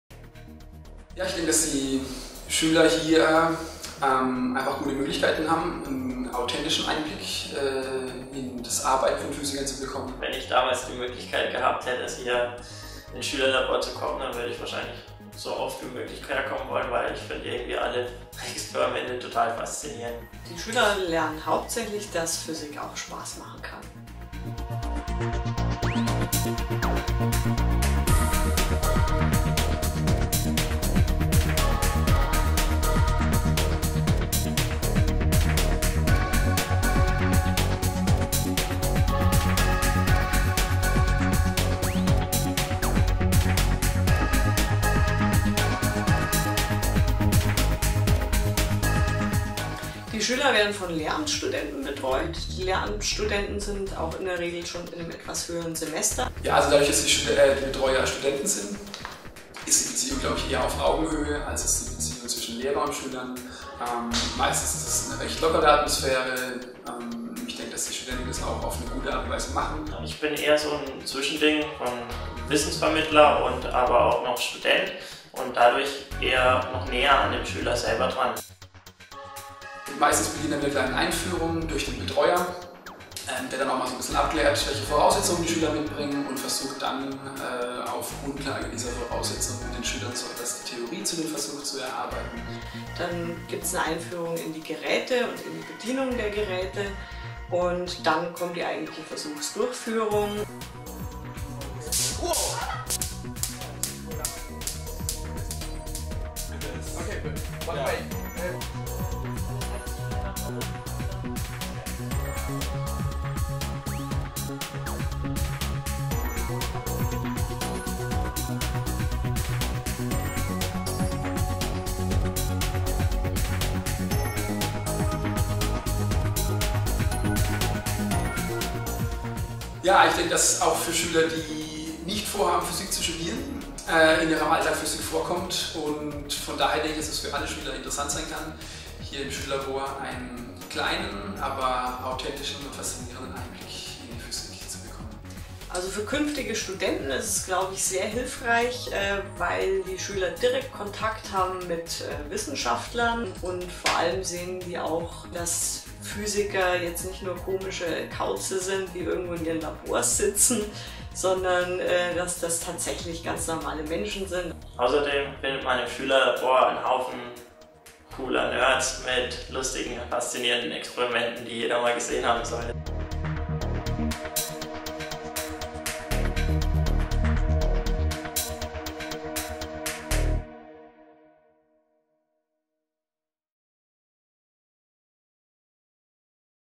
Die Physik Schülerlabor Initiative (PSI)
Die PSI am Karlsruher Institut für Technologie will SchülerInnen anhand einer Reihe von außergewöhnlichen Versuchen an faszinierenden Bereichen der Physik teilhaben lassen.
Sie versteht sich als eine Ergänzung zum Physikunterricht an der Schule, die den direkten Kontakt mit WissenschaftlerInnen ermöglicht und somit Einblicke in die Arbeitsweise an Forschungseinrichtungen gewährt.